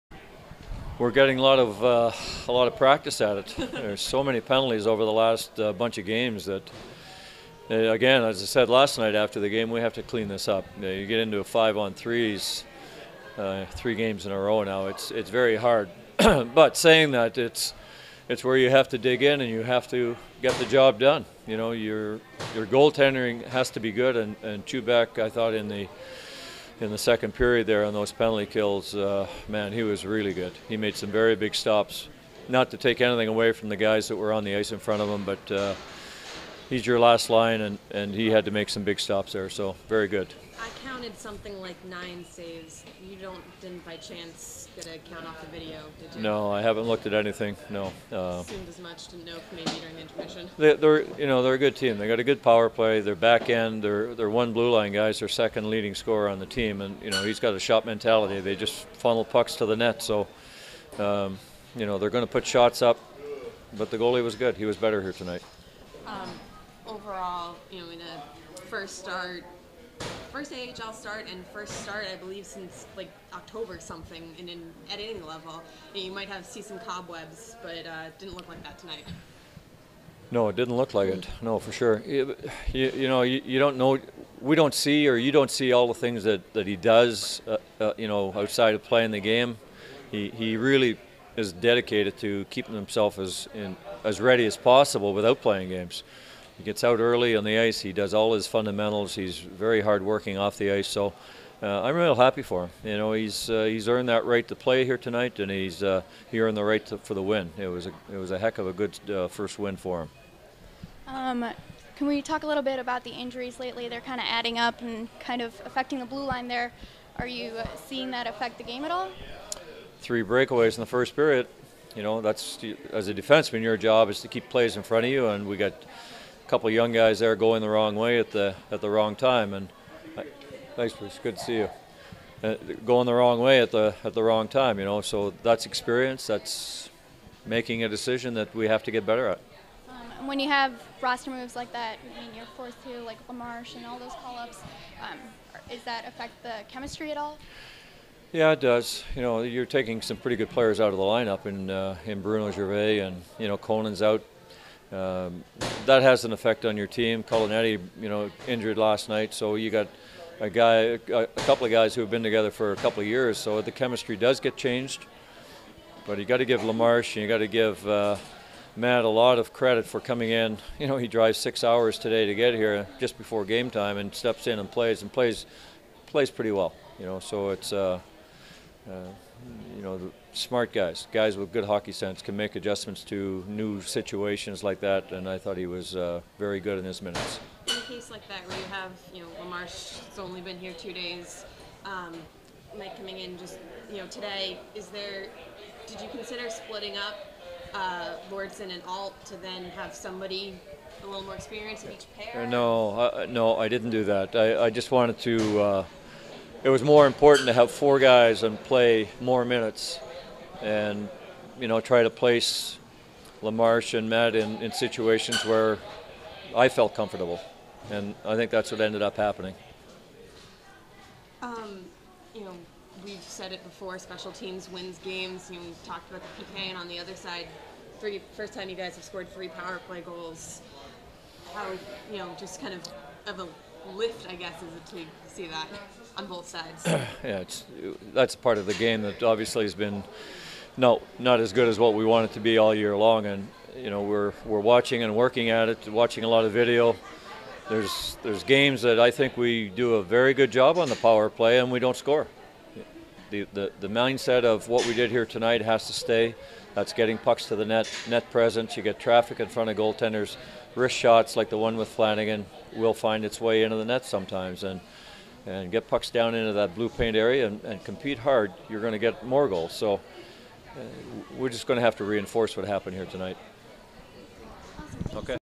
PhantomsTV! Terry 'Murray Postgame December 28, 2013
Adirondack 3 - Providence 1 on a spectacular 38-save win by rookie Carsen Chubak in his first AHL start.